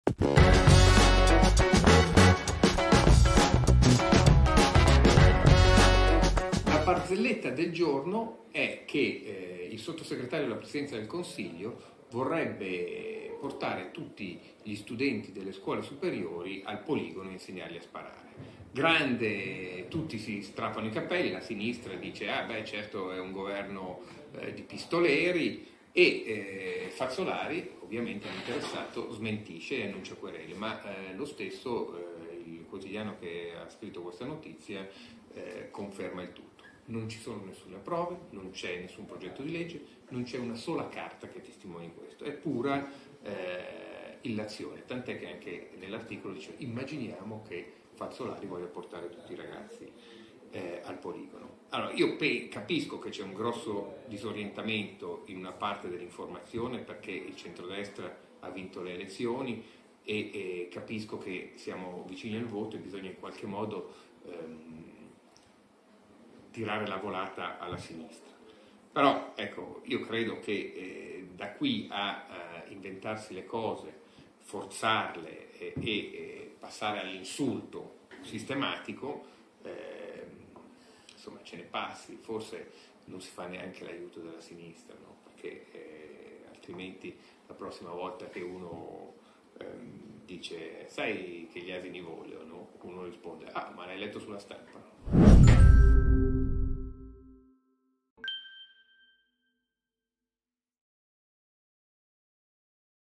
Fazzolari-pistolero, "lo hai letto sulla Stampa?": la barzelletta del giorno
Il caso del giorno. O meglio la barzelletta del giorno? Giovanbattista Fazzolari vorrebbe portare tutti i ragazzi al poligono per insegnare ai più giovani a sparare. Il tutto ovviamente è stato smentito. Una storia che conferma le difficoltà della sinistra e della loro stampa di riferimento